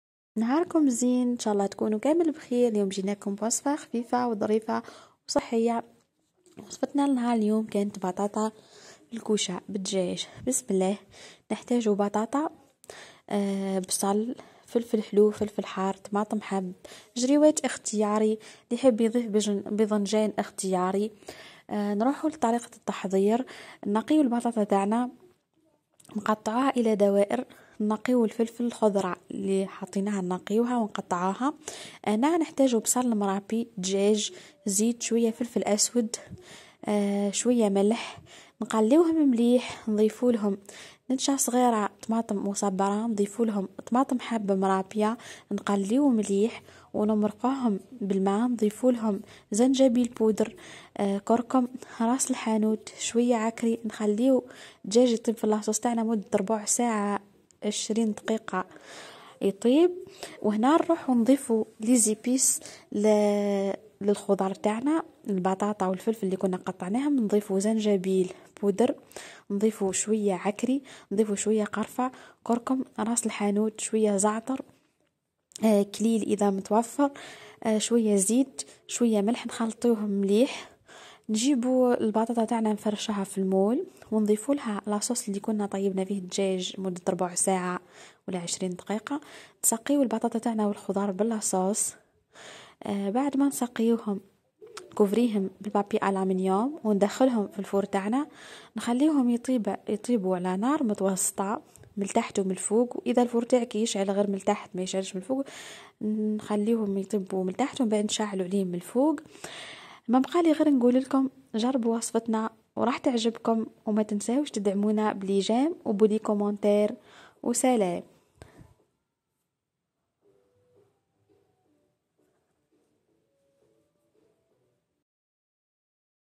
أروع بطاطا كوشة بالدجاج بنينة مراحش تشبعي منها # أطباق _رمضانية
بطاطا في الكوشة فيها وحد البنة تخليك مدمنة عليها وصفة ساهلة وصحية